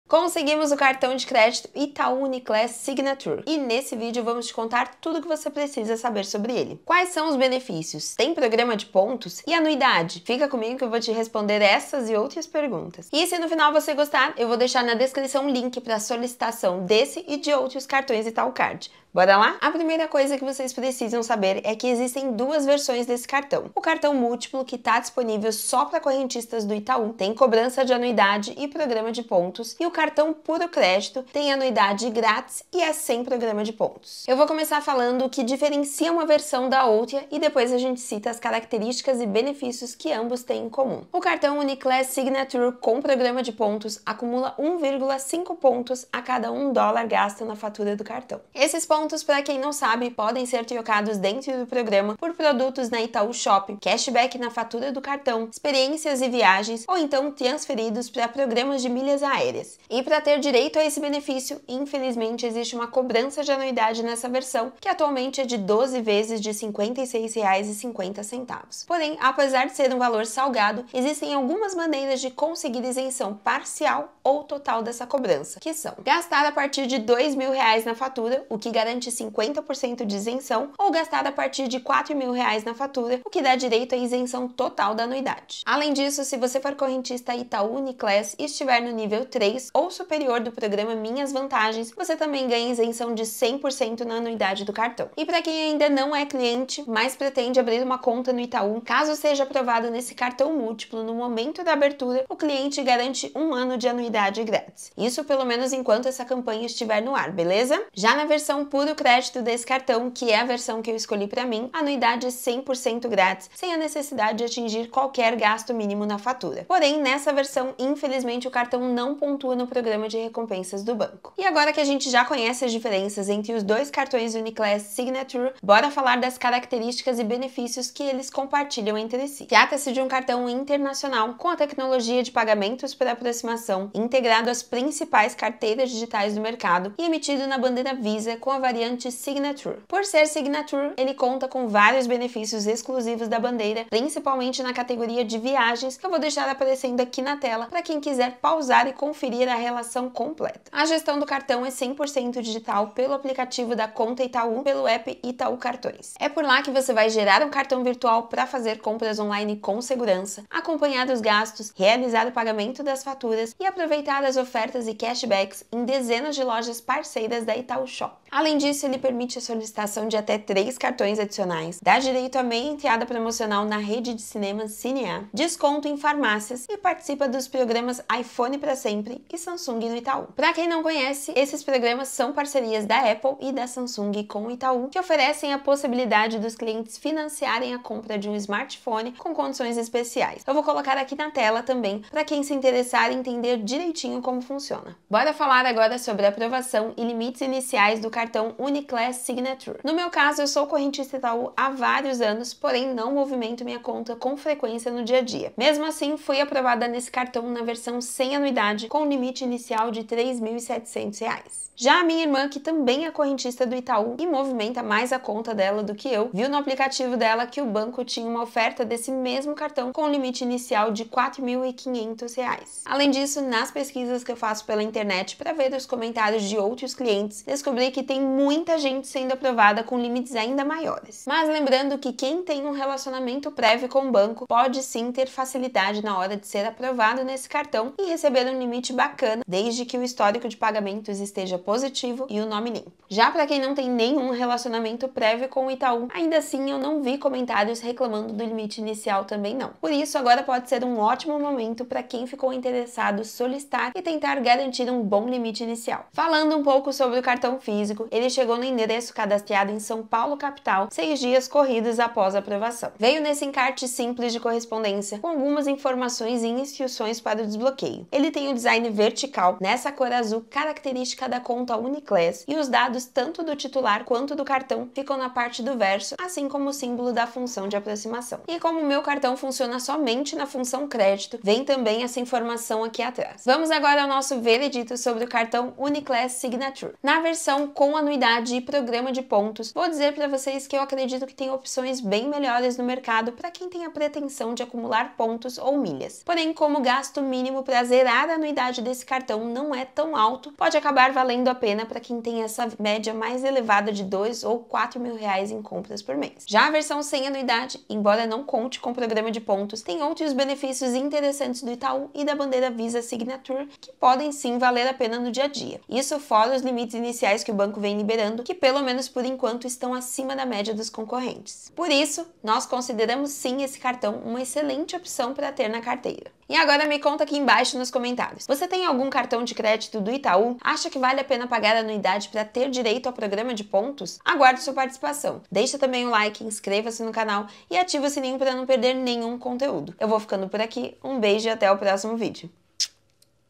Cartão de Crédito UNICLASS VISA SIGNATURE do Itaú É BOM? Pontos ou Anuidade Grátis? SAIBA TUDO 💳
Nesse vídeo eu te conto tudo sobre o cartão de crédito Itaú Uniclass Visa Signature, que é um produto porta de entrada para cartões de alta renda. Vale a pena? Quais são os benefícios? Tem programa de pontos? SAIBA TUDO!

⚠️ IMPORTANTE: Esse cartão não dá acesso a Salas VIP.